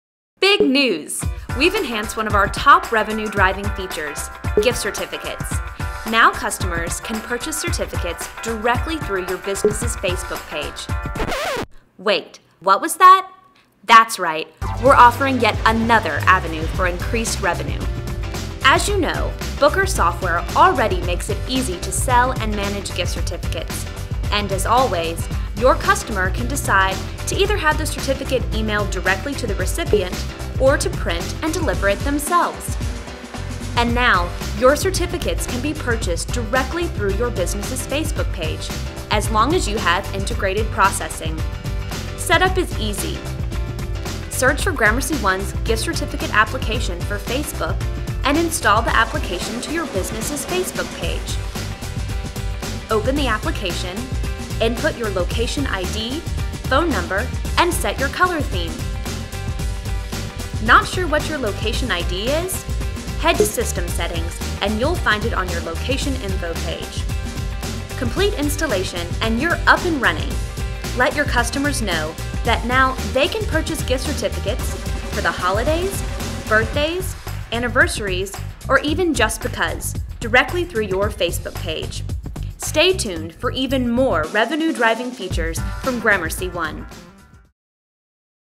Gift Certificates + Facebook
Booker software makes selling and managing gift certificates even easier. Sell gift certificates directly through your business' Facebook page with integrated processing.